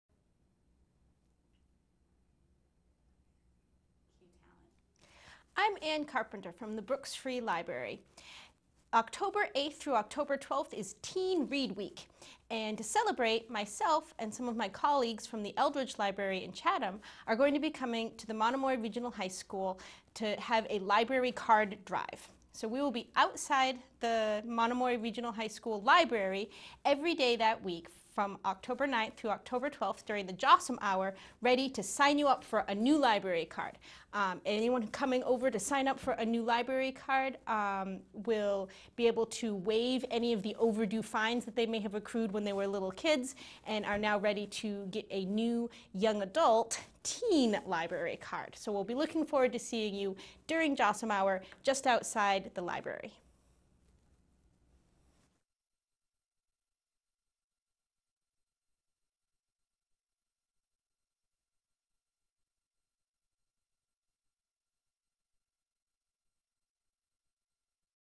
Teen Read Week 2018
Ann Carpenter talks about the upcoming Teen Read Week and how member from the local libraries with be at Monomoy Regional High School to help teens sign up for new library cards.